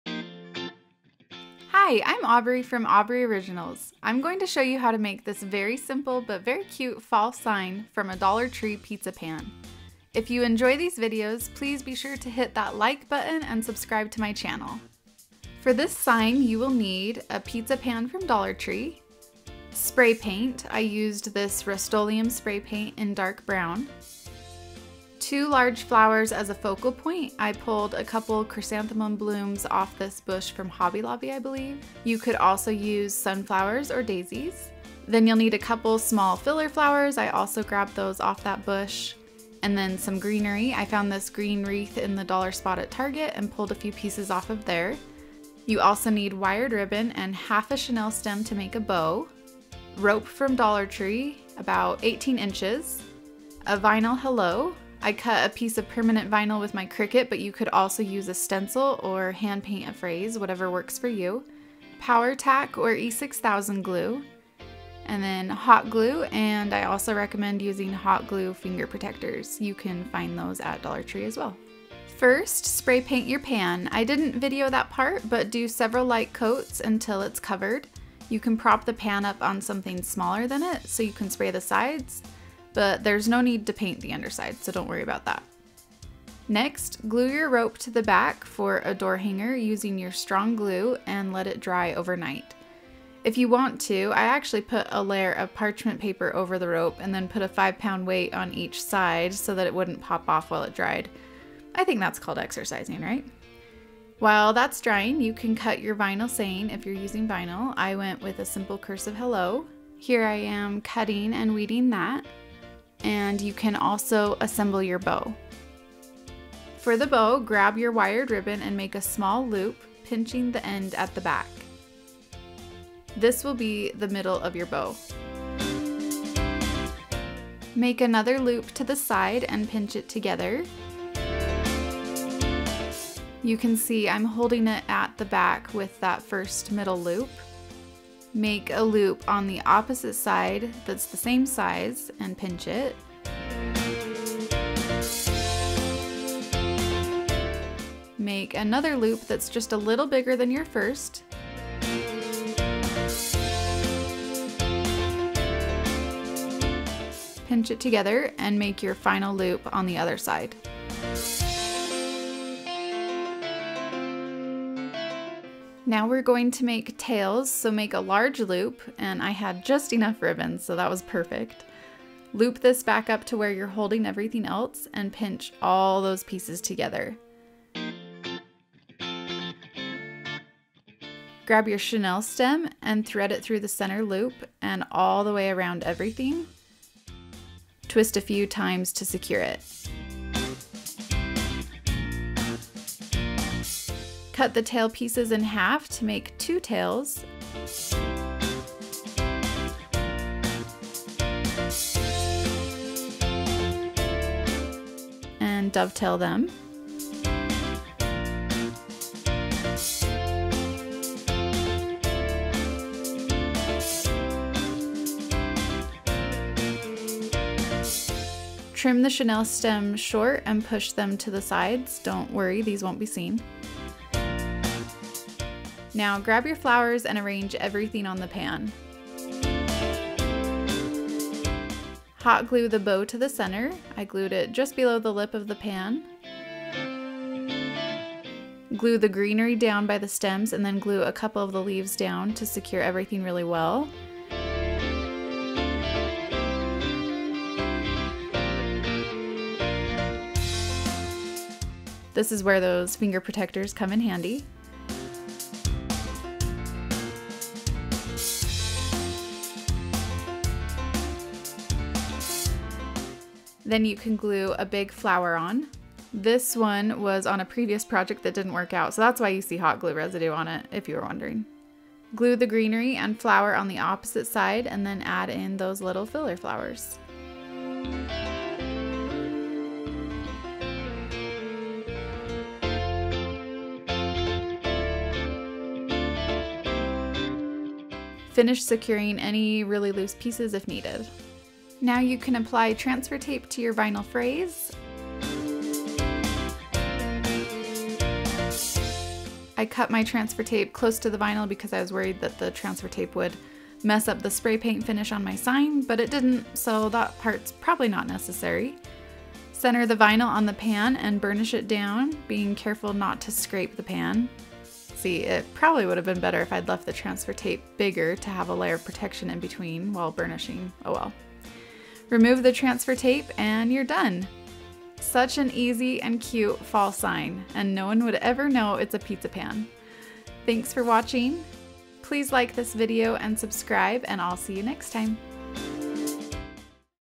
DIY Fall Pizza Pan Wreath: Easy Dollar Tree Craft
Learn how to make a cheap and easy fall wreath from a Dollar Tree pizza pan! Pizza pan crafts are so fun and they make great holiday door hangers.

00:00 DIY Fall Pizza Pan Sign
00:15 Supplies
01:12 Step 1: Paint the Pizza Pan
01:26 Step 2: Make a Rope Wreath Hanger
01:44 Step 3: Cut a Vinyl Saying
01:53 Step 4: Make a Ribbon Bow
03:45 Step 5: Assemble the Pizza Pan Sign
05:30 Please like and subscribe!

spray paint
faux flowers
faux greenery
a vinyl saying
E6000 glue (I used Power-Tac in the video, but I actually prefer E6000)